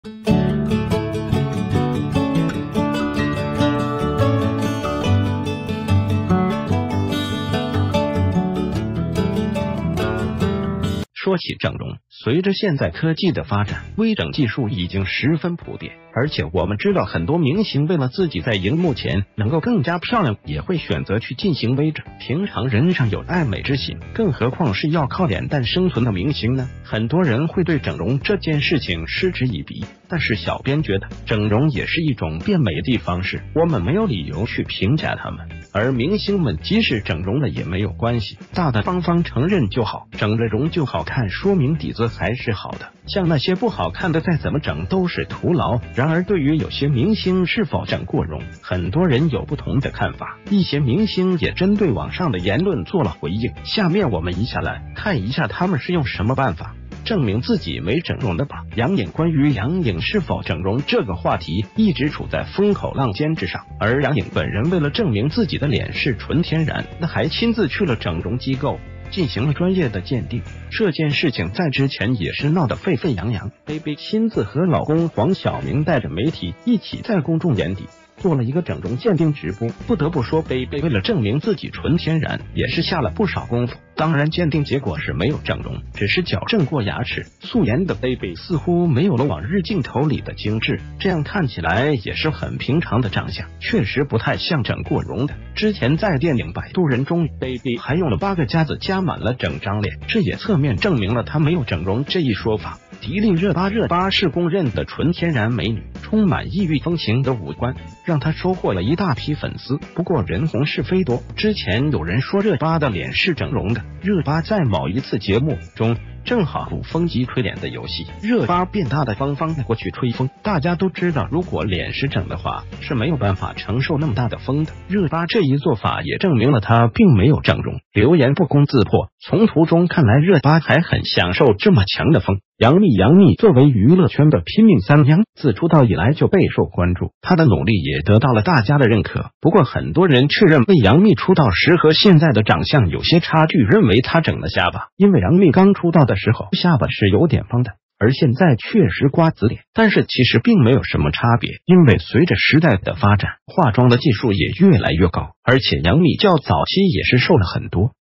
女星為證自己純天然煞費苦心，熱巴吹風機吹臉，楊穎沒人敢學
說起整容，隨著現在科技的發展，微整技術已經十分普遍。而且我們知道很多明星為了自己在熒幕前能夠更加漂亮也會選擇去進行微整。平常人尚有愛美之心，更何況是要靠臉蛋生存的明星呢。很多人會對整容這件事情嗤之以鼻，但是小編覺得，整容也是一種變美的方式，我們沒有理由去評價他們，而明星們即使整容了也沒有關系，大大方方承認就好，整了容就好看說明底子還是好的，像那些不好看的再怎么整都是徒勞。然而對于有些明星是否整過容，很多人有不同的看法，一些明星也針對網上的言論做了回應，下面我們一下來看一下他們是用什么辦法證明自己沒整容的吧。楊穎關于楊穎是否整容這個話題一直處在風口浪尖之上。而楊穎本人為了證明自己的臉是純天然的還親自去了整容機構進行了專業的鑒定，這件事情在之前也是鬧得沸沸揚揚，baby親自和老公黃曉明帶著媒體一起在公眾眼底做了一個整容鑒定直播，不得不說baby為了證明自己純天然也是下了不少功夫。當然鑒定結果是沒有整容，只是矯正過牙齒。素顏的baby似乎沒有了往日鏡頭里的精致，這樣看起來也是很平常的長相，確實不太像整過容的。之前在電影《擺渡人》中baby還用了八個夾子夾滿了整張臉，這也側面證明了她沒有整容這一說法。迪麗熱巴熱巴是公認的純天然美女，充滿異域風情的五官讓她收獲了一大批粉絲，不過人紅是非多，之前有人說熱巴的臉是整容的，熱巴在某一次節目中正好鼓風機吹臉的游戲，熱巴便大大方方的過去吹風，大家都知道如果臉是整的話是沒有辦法承受那么大的風的，熱巴這一做法也證明了她并沒有整容，流言不攻自破。從圖中看來熱巴還很享受這么強的風。楊冪楊冪作為娛樂圈的拼命三娘，自出道以來就備受關注，她的努力也得到了大家的認可。不過很多人確認為楊冪出道時和現在的長相有些差距認為她整了下巴，因為楊冪剛出道的時候下巴是有點方的，而現在確實瓜子臉。但是其實并沒有什么差別，因為隨著時代的發展，化妝的技術也越來越高，而且楊冪較早期也是瘦了很多，瘦了下來自然而然臉也就瘦了，所以傳言也就不攻自破了。其實對于明星是否整容這件事情大家也不必太過關心，因為現在有誰不進行個微整，平常人皆有愛美之心，更何況這些天天在鎂光燈下的明星呢。